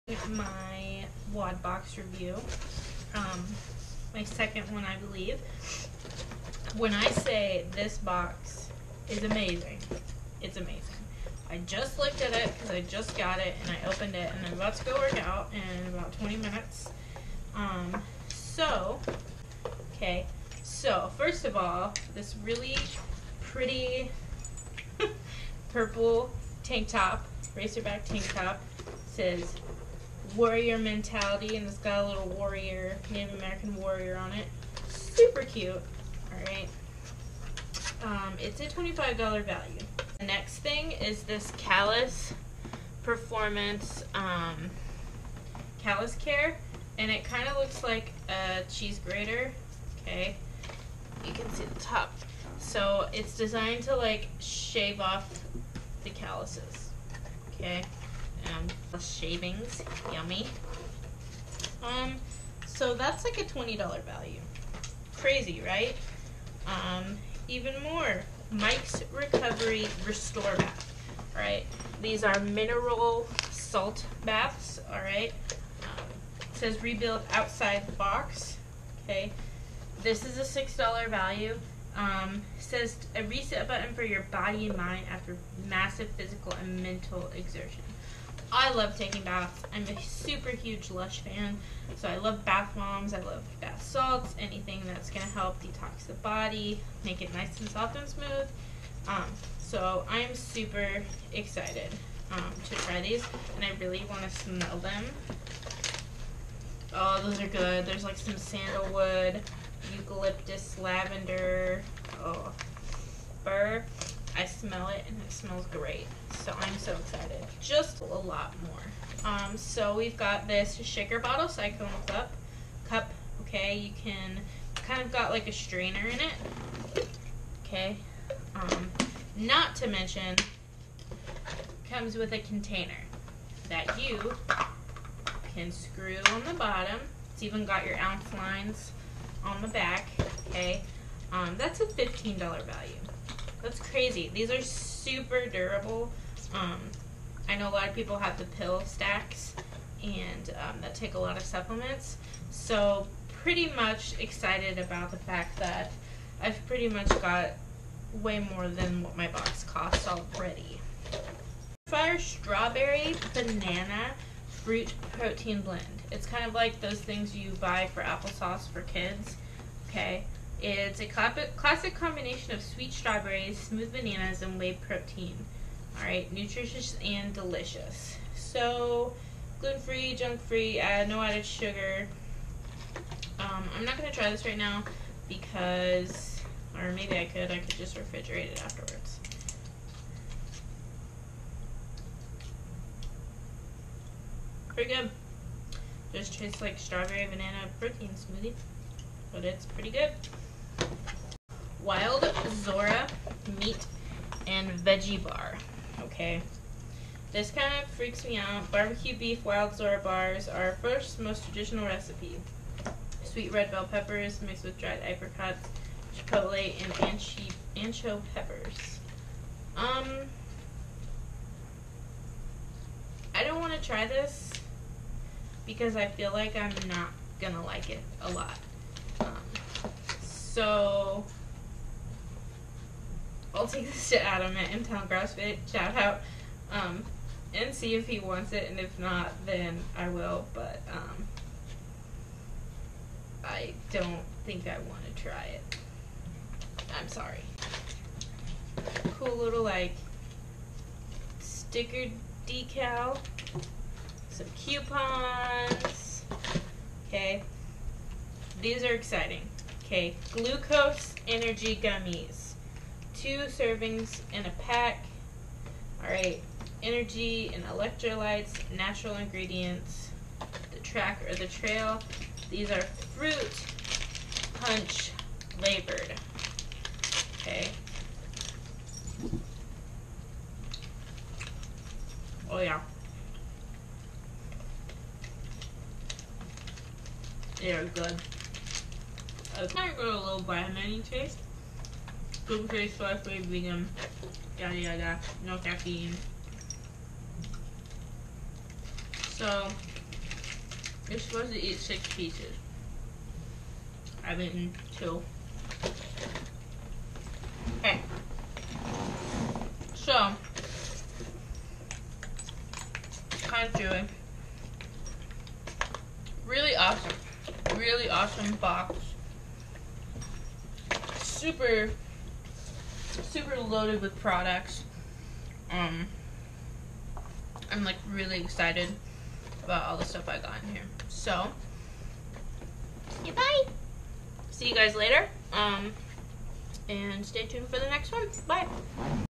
Wodbom2
WodBom subscription box review, a real person doing a real review!